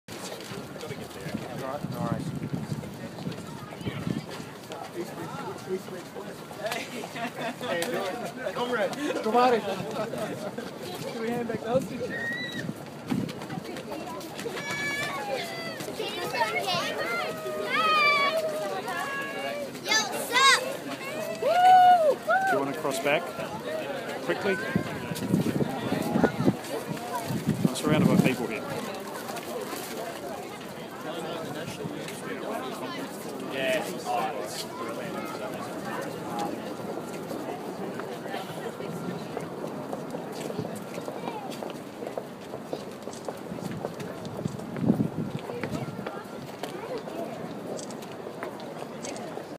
New bridge open for business
891's Brekky team were on the new footbridge across the Torrens as the public take their first steps on one of Adelaide's most hotly debated pieces of kit.